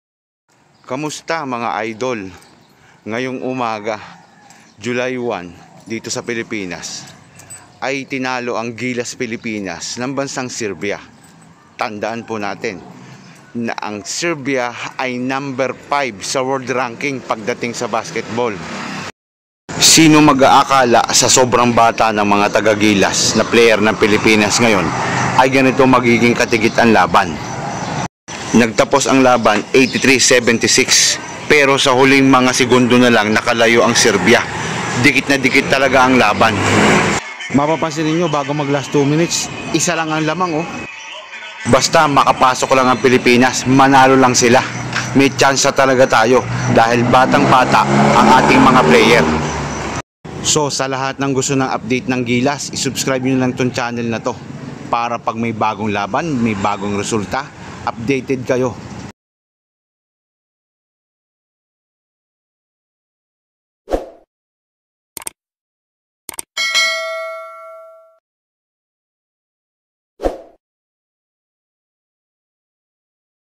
Gilas vs Serbia Pinahirapan ng Pilipinas ang Serbia Magandang Laban hanggang Huli
Muntikan Pang talunan ng Gilas Pilipinas ang World #5 na serbia sa kakatapos na laro.
Nag tapos ang laro 83 - 76